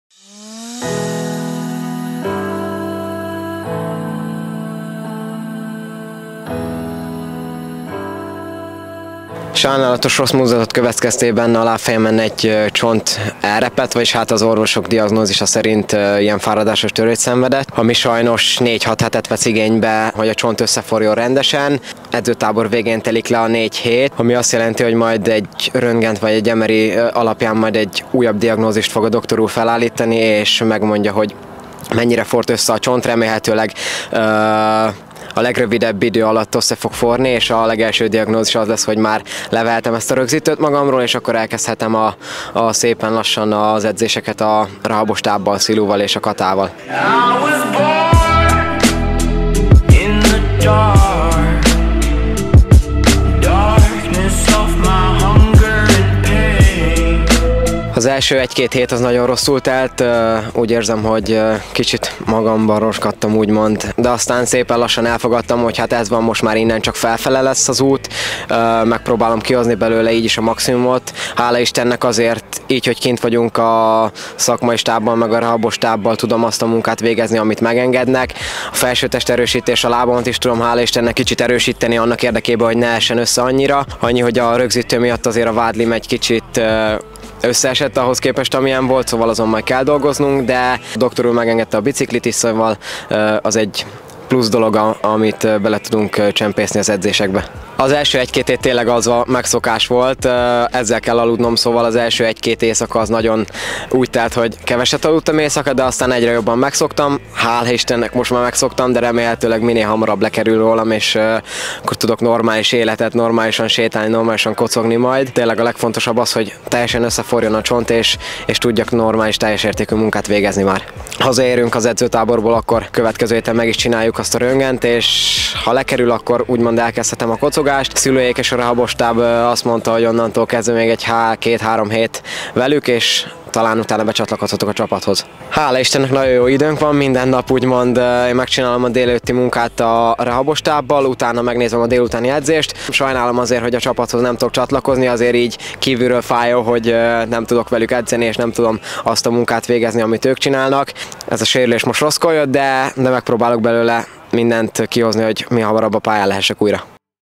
Rosszkor jött a sérülés
Az első néhány éjszakát megszenvedte lábtörése után Kiss Tamás, aki reméli, hamarosan a többiekkel edzhet.